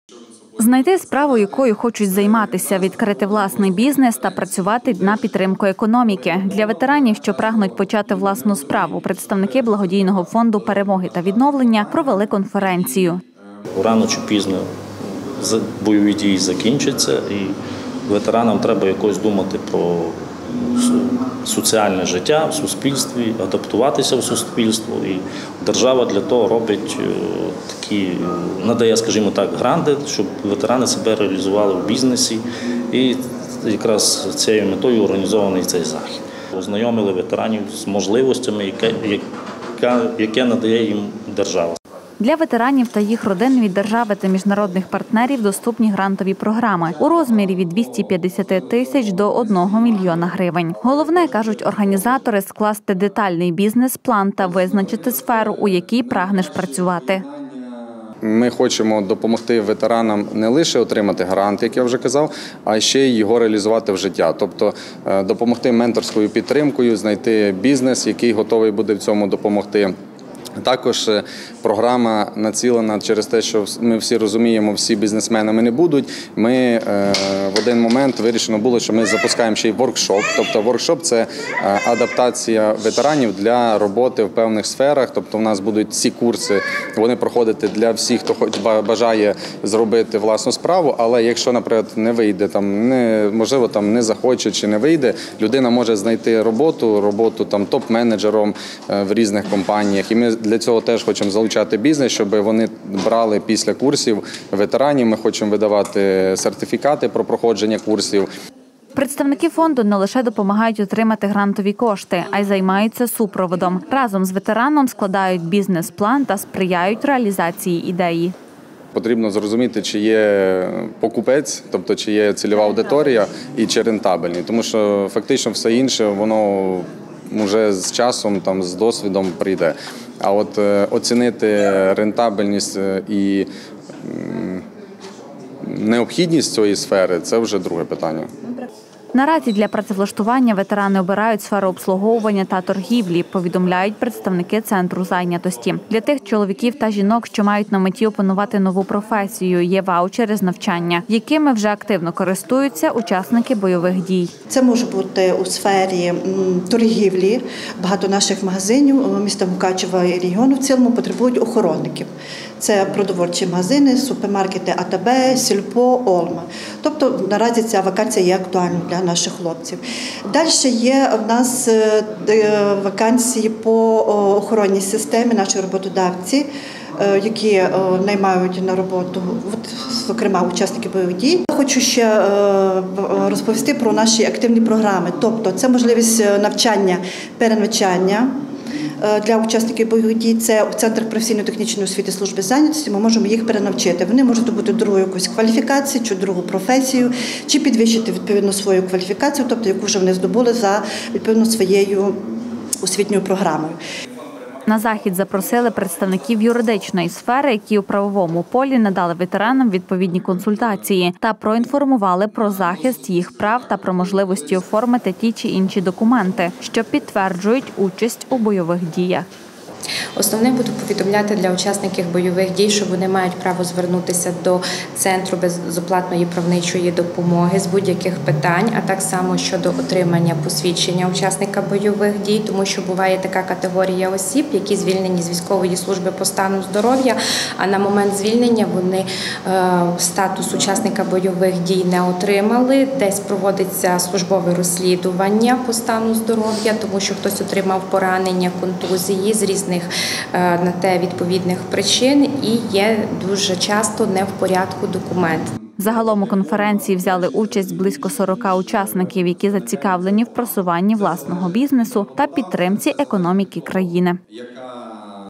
Бізнес для ветеранів
Знайти справу, якою хочуть  займатися, відкрити власний бізнес та працювати для підтримки економіки – для ветеранів, що прагнуть почати власну справу представники благодійного фонду «Перемоги та відновлення» провели конференцію. Для ветеранів та їх родин від держави та міжнародних партнерів  доступні грантові програми у розмірі від 250 тисяч до 1 мільйона гривень. Головне, кажуть організатори, скласти детальний бізнес-план та визначити сферу у якій прагнеш працювати. Представники фонду не лише допомагають отримати грантові кошти, а й займаються супроводом, разом з ветераном складають бізнес план та сприяють реалізації ідеї .Наразі для працевлаштування ветерани обирають сферу обслуговування та торгівлі, повідомляють  представники Центру зайнятості. Для тих чоловіків та жінок, що мають на меті опанувати нову професії є ваучери з навчання, якими вже активно користуються учасники бойових дій. На захід запросили працівників юридичної сфери, які у правовому полі надали  ветеранам відповідні консультації та проінформували про захист їх прав та про можливість оформити ті чи інші документи, що підтверджують участь у бойових діях. Загалом у конференції взяли участь близько 40- ка захисників, які зацікавлені у просуванні власного бізнесу та підтримці економіки країни.